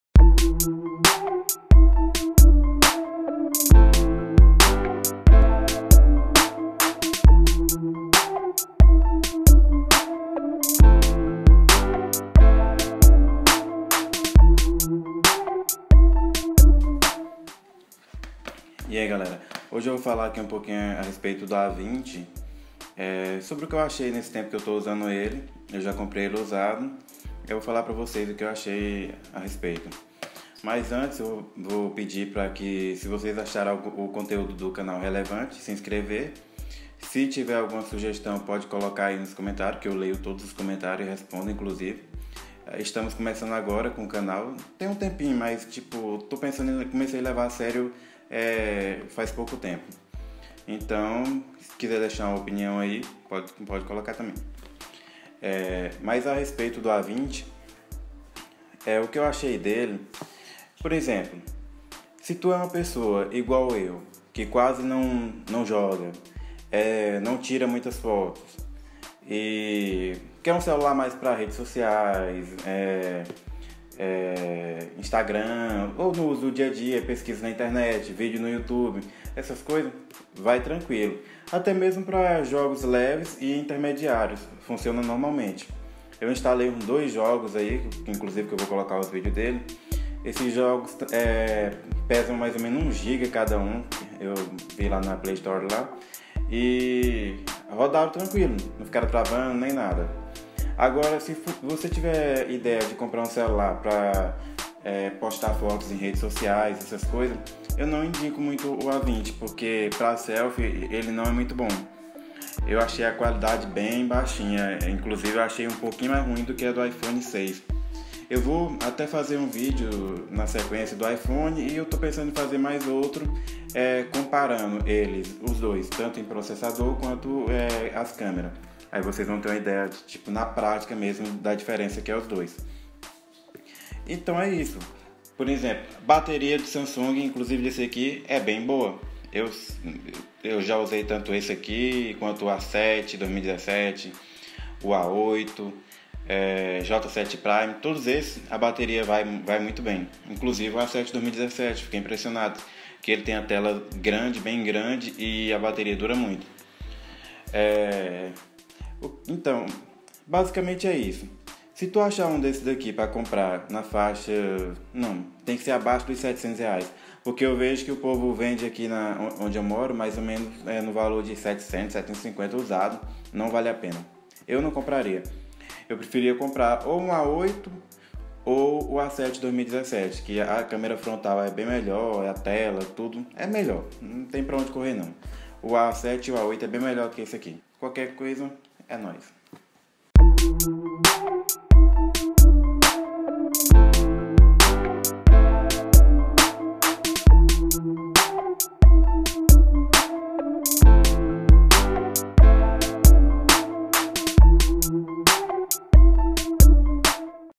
VALE A PENA GALAXY A20 EM 2020 ? opinião de usuário
fala galera!

Segue um vídeo rápido falando minha opinião de usuário a respeito do galaxy A20, como eu sempre digo, minha opinião é levando em conta que o aparelho é usado. Isso não é um review,  apenas digo se eu acho que vale ou não a pena ter e em que faixa de preço.